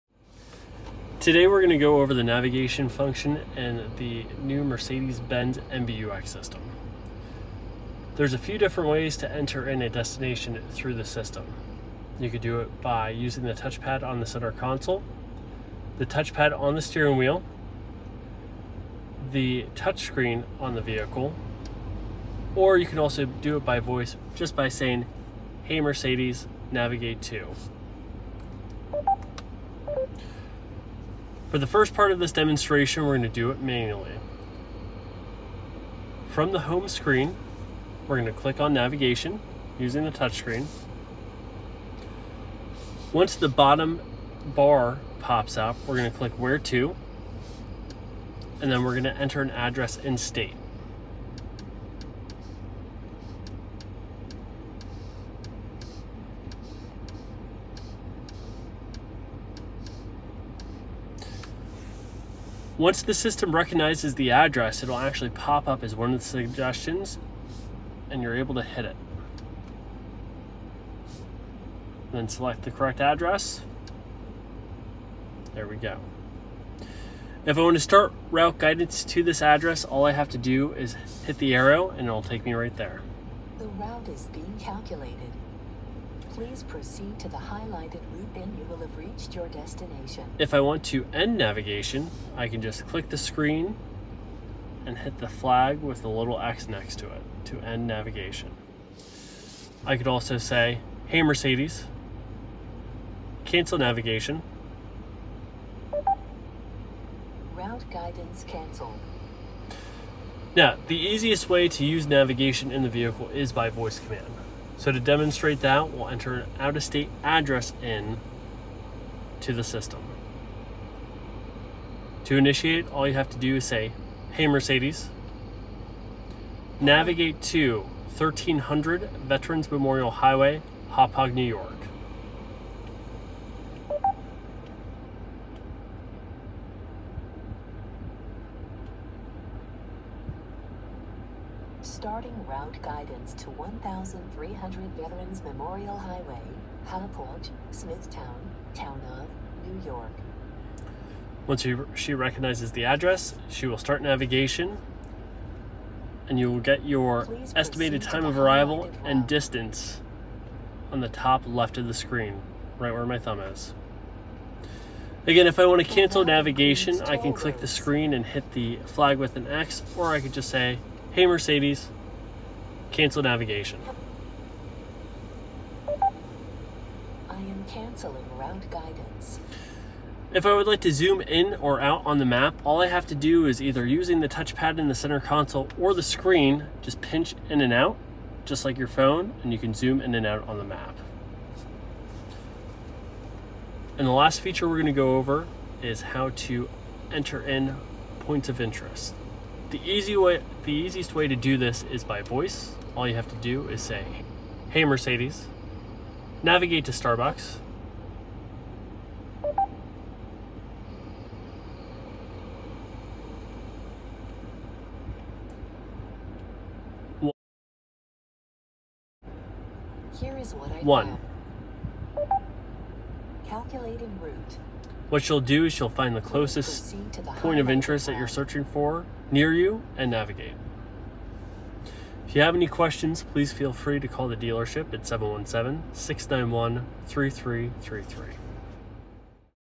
Mercedes Benz MBUX Navigation Tutorial
In this video we will go over the new Mercedes MBUX system. This system is in most 2020 and 2021 models including GLS, GLE, GLC, GLB, GLA, E-Class, and A-Class. We will be going over how to enter destinations with the touch screen and voice command system.

*This video was filmed in a 2021 GLE 350